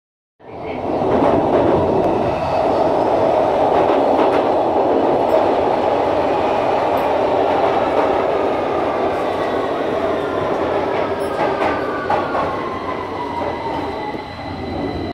Dominik's first scary time on the underground
My baby boy's first time to travel on underground in London